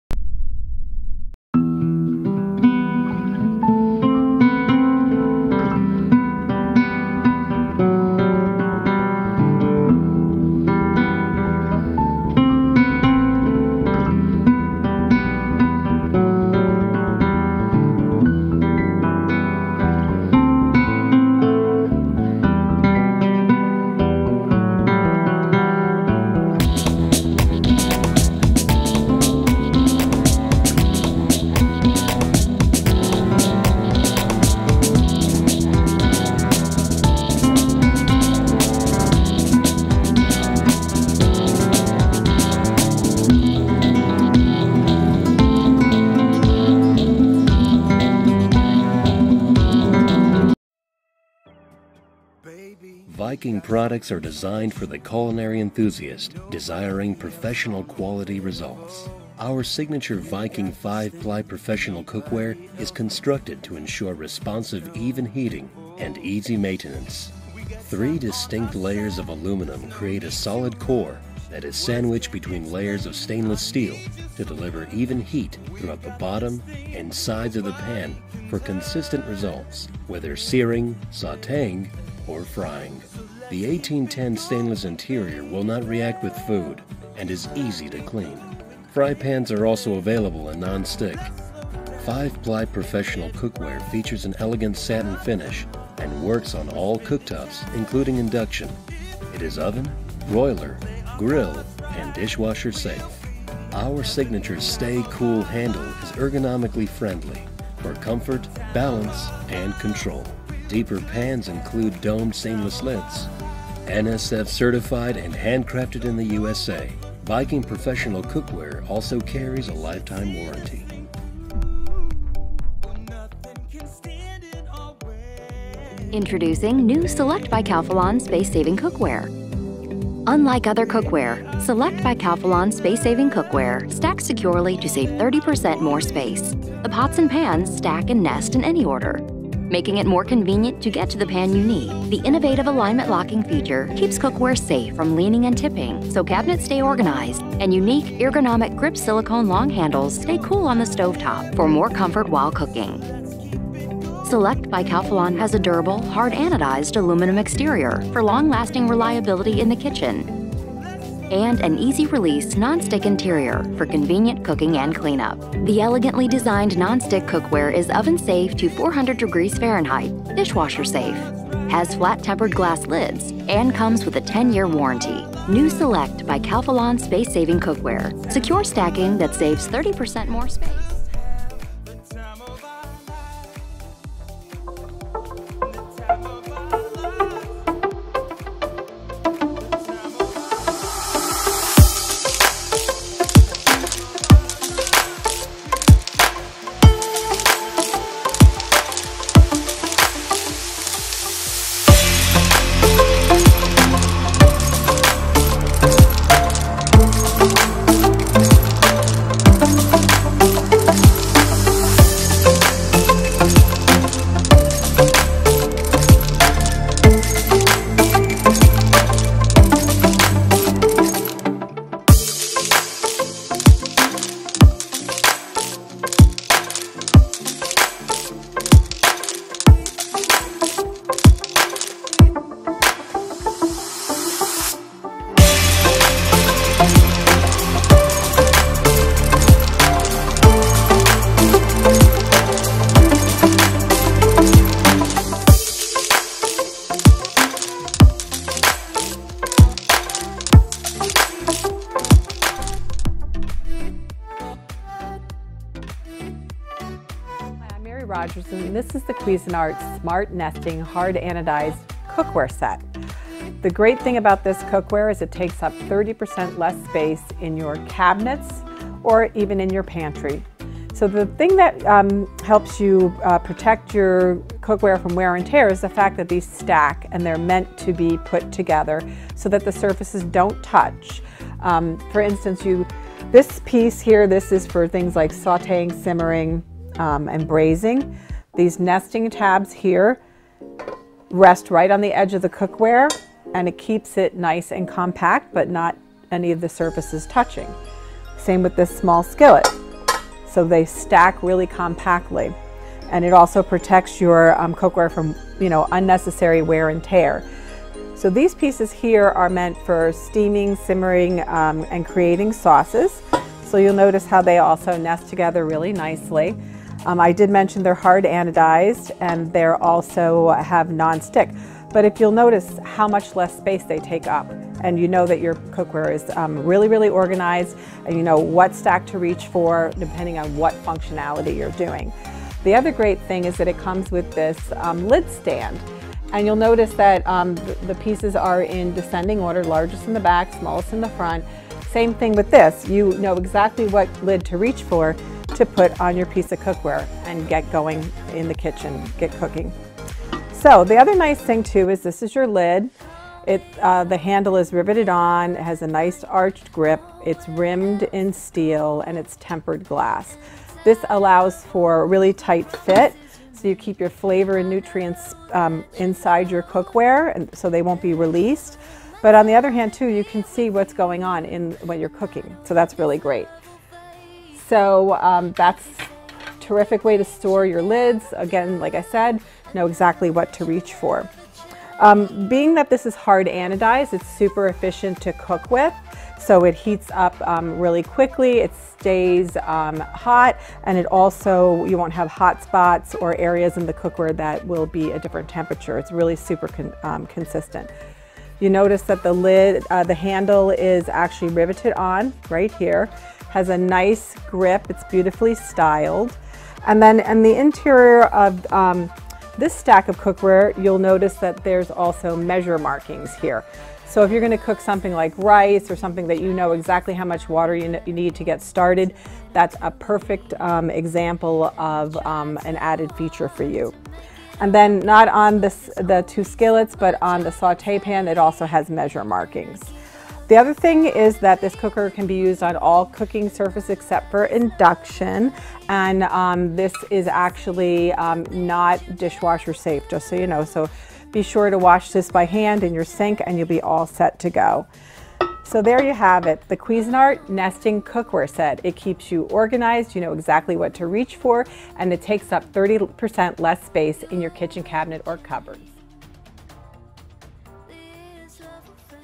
5 Best Cookware Set 2021
5? Le Creuset Toughened Nonstick PRO Cookware Set
??Amazon US

4? Viking Professional 5-Ply Stainless Steel Cookware Set
??Amazon US

3? Select by Calphalon Space-Saving Hard Anodized Nonstick Pots and Pans, 14-Piece Cookware and Utensil Set
??Amazon US

2? Tramontina Monaco 7pc. Cookware Set
??Amazon US

1? Cuisinart SmartNest Hard Anodized 11 Piece Cookware Set
??Amazon US